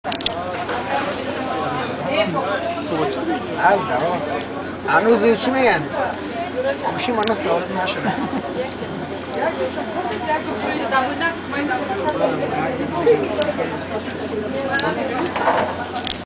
afghan nowrouz haft may-waa
Junaid jaan and Fawad jaan preparing pistachios for nowrouz haft may-waa